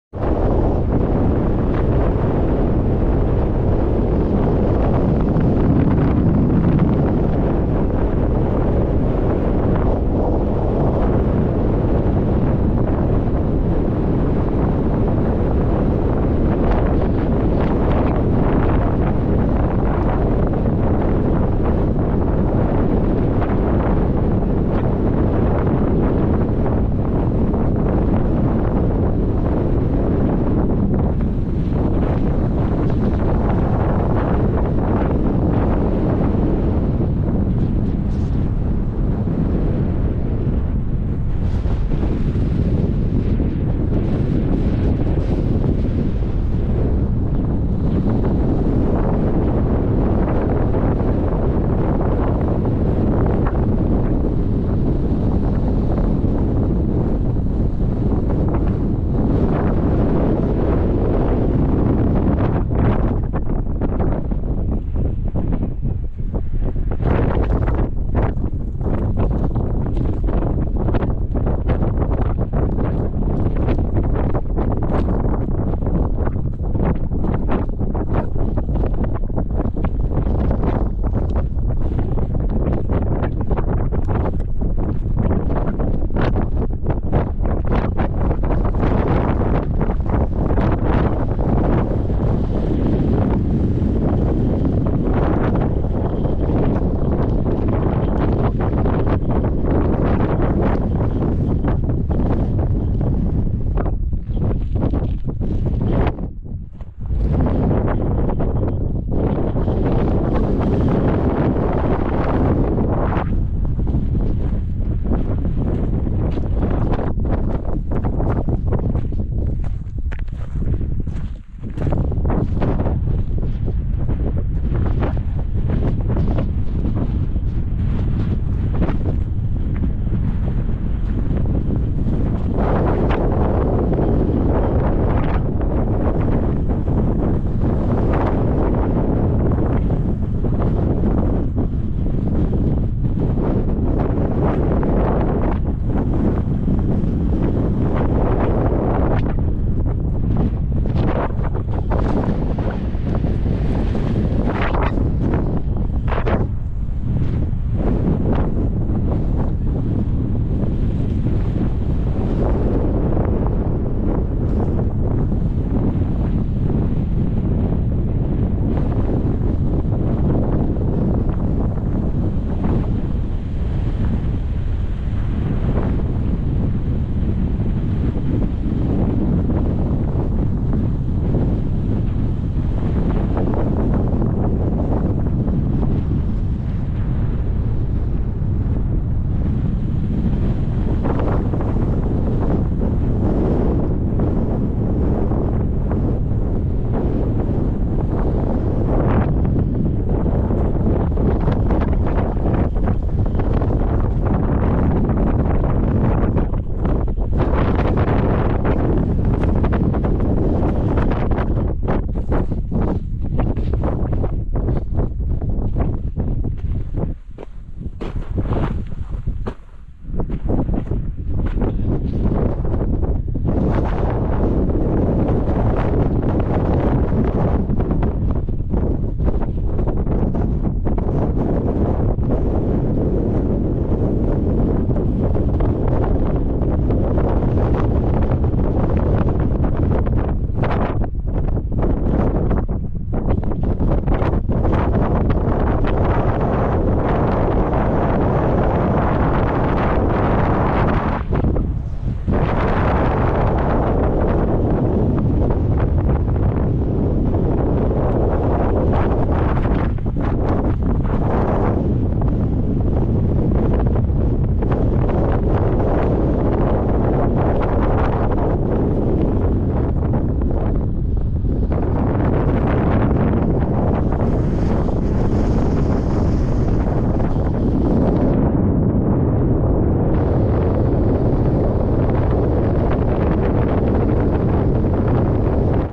Hiking next to erupting volcano! Meradalir Eruption, Iceland! 🌋 12.08.22 4K
Exploring the area around Meradalir Volcano at night on August 12th, 2022. Filmed with GoPro 10 in 4K.